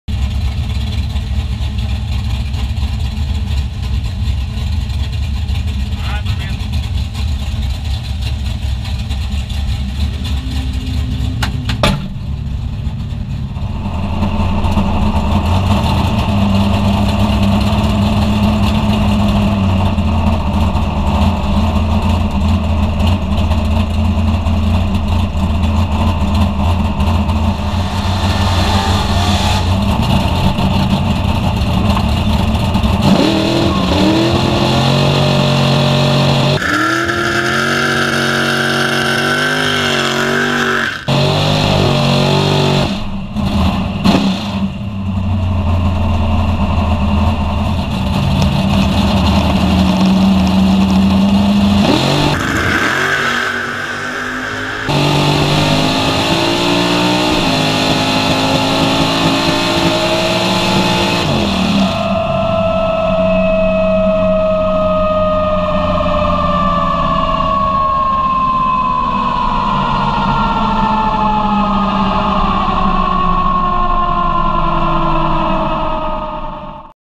Plattinum Racing Team Camaro | Bristol Dragway
A unique look at Plattinum Products' Drag Racing Team at Bristol Dragway for the BTE World FootBrake Challenge IX. Shot 100% GoPro